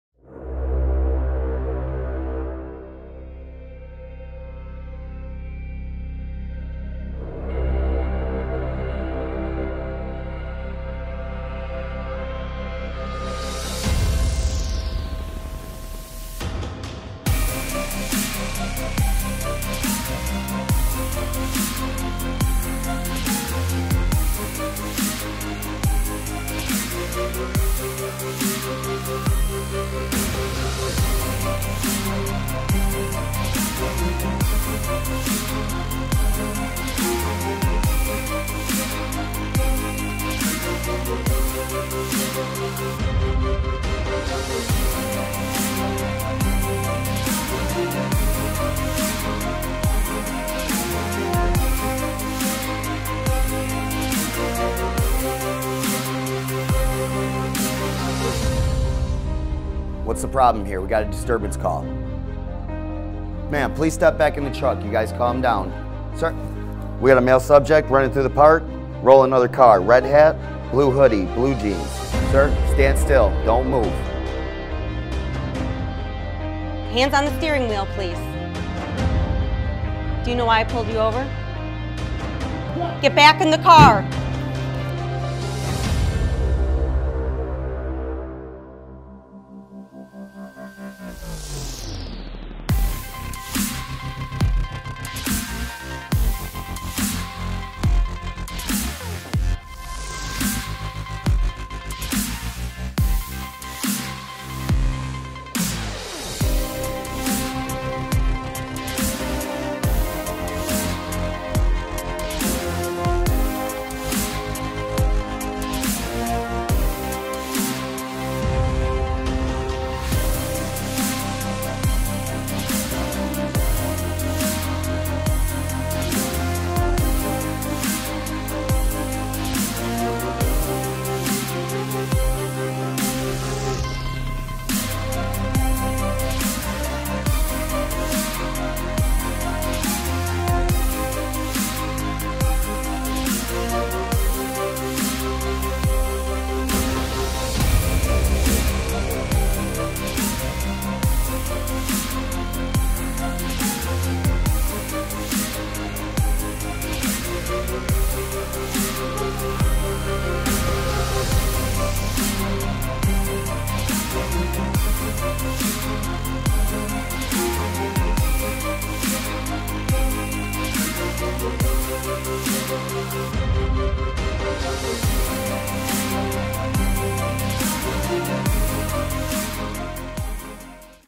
What Police Training Should Look Like
Train Police officers on situational awareness, judgment skills, critical incidents, de-escalation with a MILO Range Simulator.